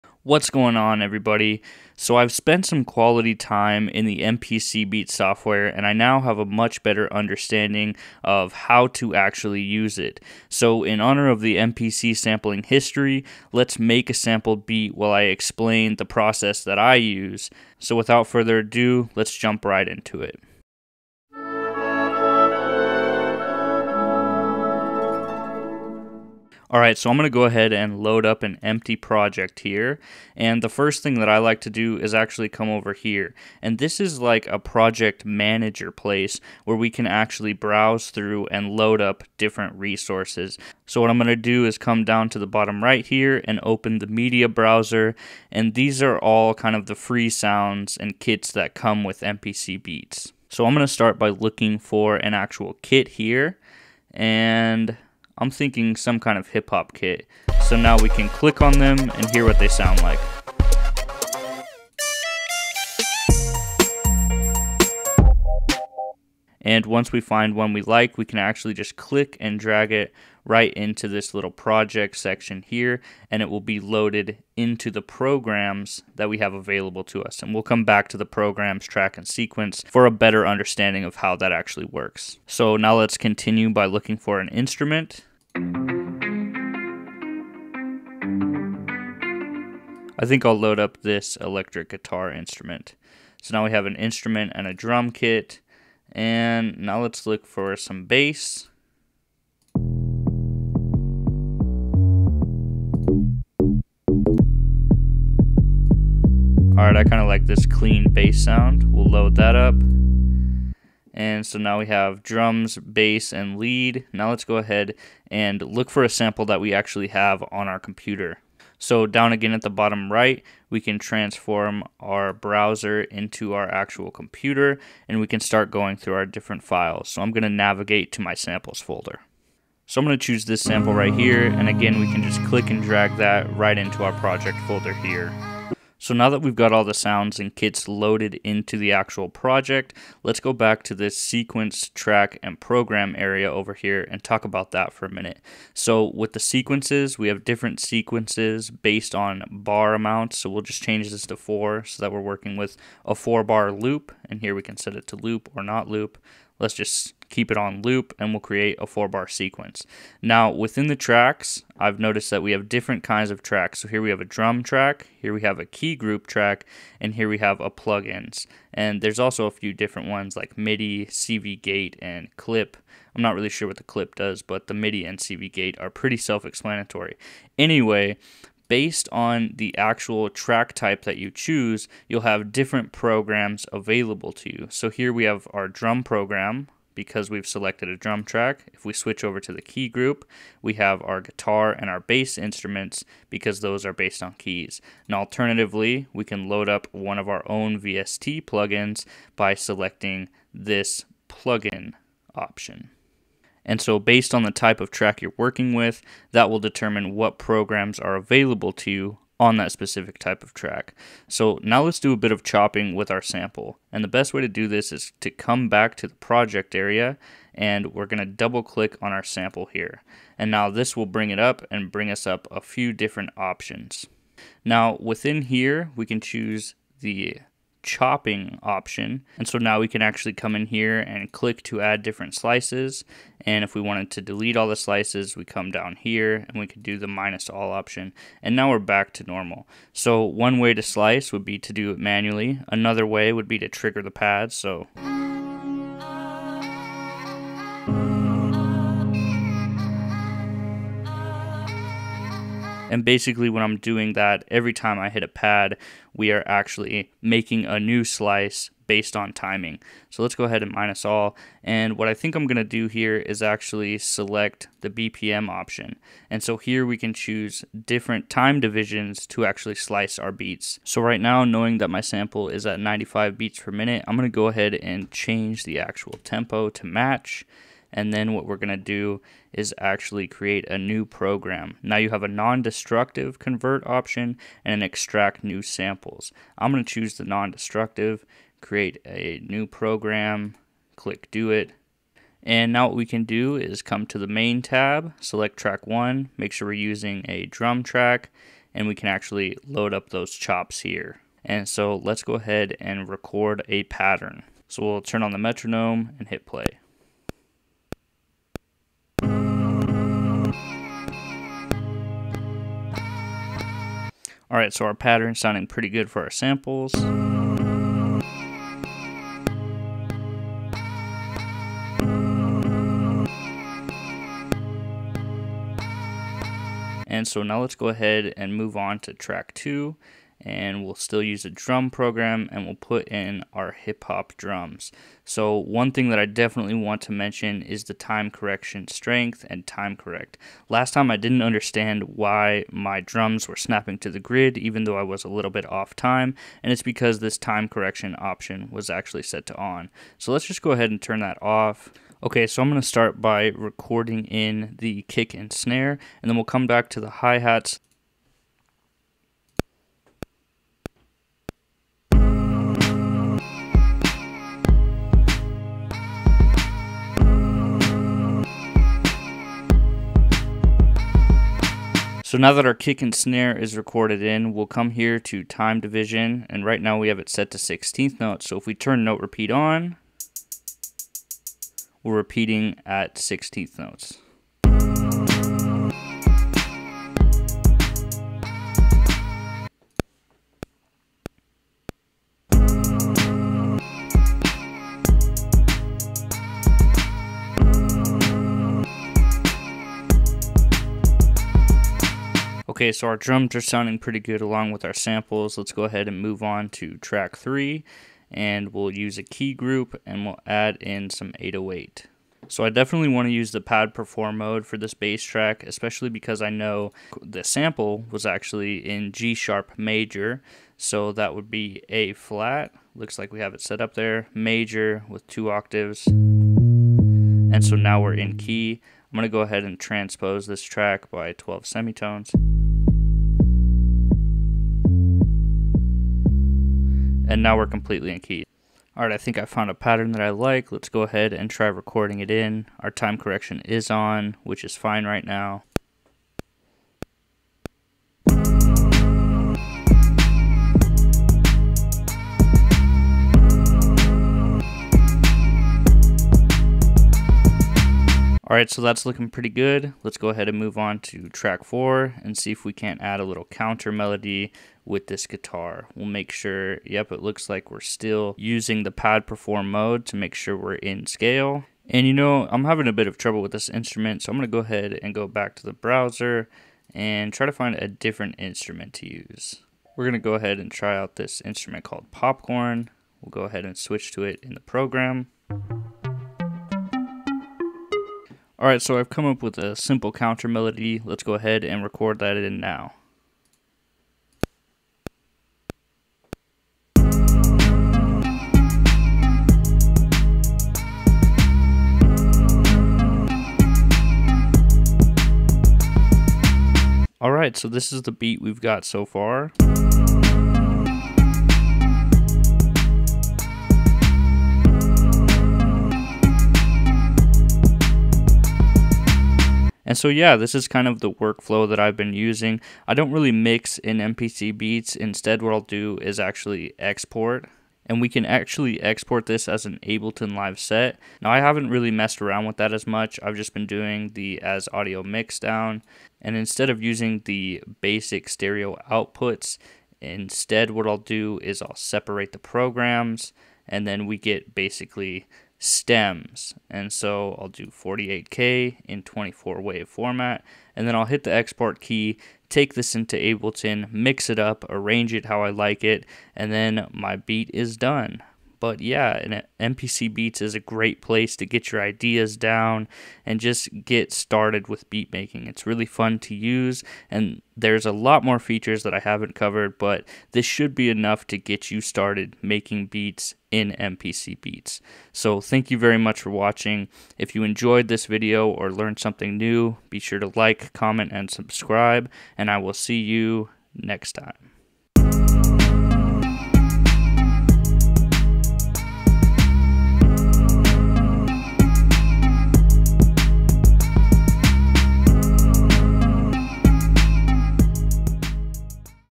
MPC Beats in UNDER 10 Minutes
🎛️ Unlock the Power of MPC Beats – Fast and Free

Dive into Akai Pro's MPC Beats, a free digital audio workstation (DAW) that brings the legendary MPC workflow to your computer. In this concise tutorial, we'll cover everything you need to start creating beats in under 10 minutes.

🔍 Key Features Covered:
16-Pad Grid: Trigger drum samples and loops with precision.
Piano Roll: Record and edit MIDI notes effortlessly.
Sample Editor: Chop and edit samples to craft unique sounds.
Sound Browser: Quickly search and select from a vast library of sounds.
Track Navigation: Easily switch between tracks and manage your project.

💡 Why Choose MPC Beats?
Comprehensive Tools: From beat creation to mixing and exporting, MPC Beats has you covered.
Rich Sound Library: Access 2GB of content, including samples, loops, and built-in virtual instruments.
Extensive Effects: Utilize over 80 audio FX plugins to shape your sound.
Third-Party Support: Expand your sonic palette with VST/AU plugin compatibility.
Live Recording: Record instruments or vocals with 2 stereo audio tracks to add live elements to your music.

Have questions or want to share your own tips about MPC Beats?